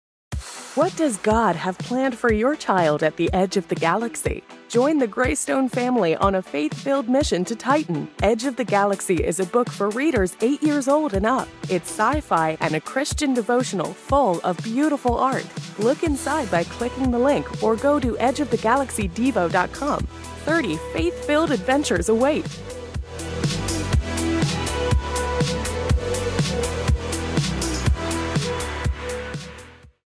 Edge of the Galaxy is HERE (Book Trailer)
A galactic adventure! Join the Greystones as they venture to Titan!

Edge of the Galaxy combines action-packed science fiction with faith-filled lessons that readers can apply to their own lives. It is a unique science-fiction devotional where you follow the exciting exploits of Gavin and his family through his star log entries. There are beautiful color illustrations throughout the book that bring this interplanetary adventure to life!

The year is 2095, and 12-year-old Gavin Greystone is on the space station Provider above the Saturn moon of Titan. In Edge of the Galaxy, Gavin and his family work and live as space explorers. Life in space is certainly not easy, or even all that safe, but it’s definitely an exciting adventure. From Provider, Gavin’s family will work with other families to create a life on the surface of Titan as they expand humankind’s footprint and prepare for more settlers. Through many trials, Gavin will learn who he is and the plan God has for him.